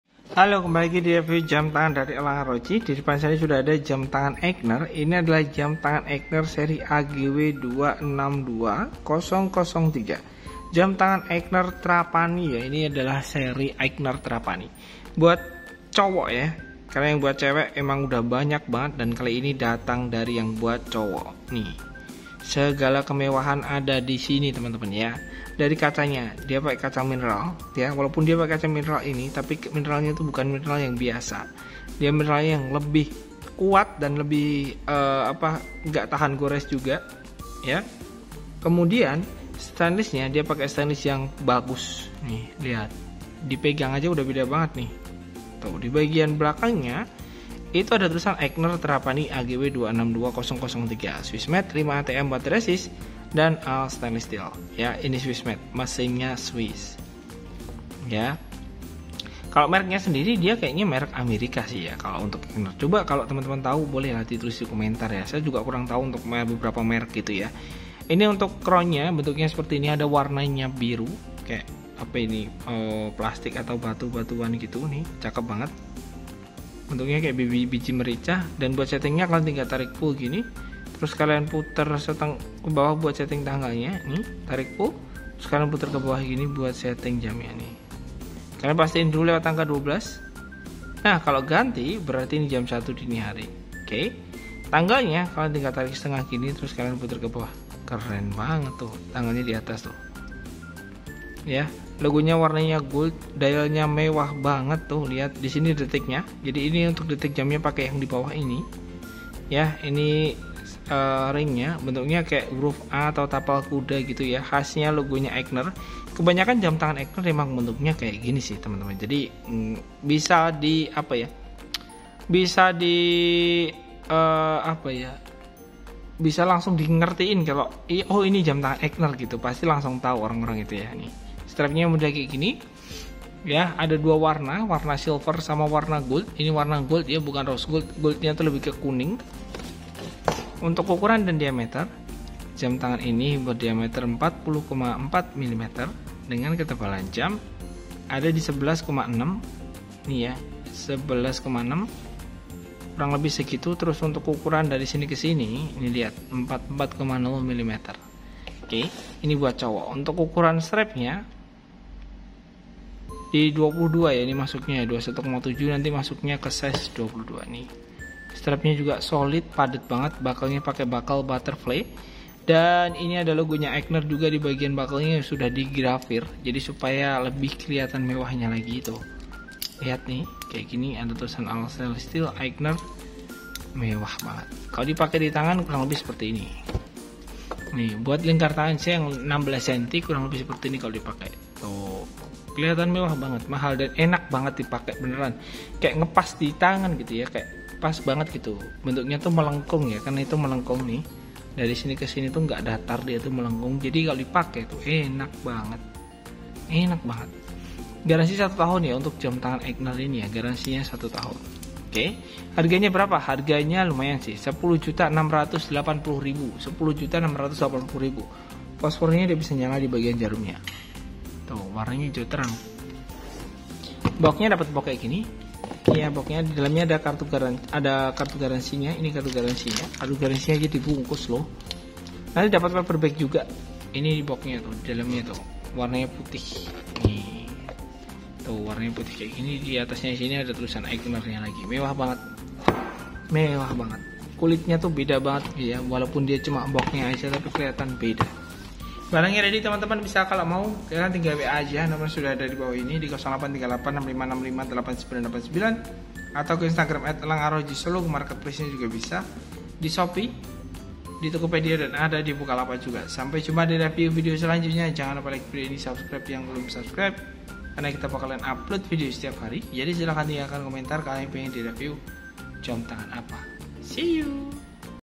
AIGNER AGW.262003 : JAM TANGAN AIGNER MEWAH BANGET SWISS MADE TERBARU (2024)
REVIEW DAN DETAIL JAM TANGAN AIGNER AGW.262003 TERBARU.

Belanja Online?

Just contact =
ELANG ARLOJI
Jl. Kebangkitan Nasional No 34
Solo 57141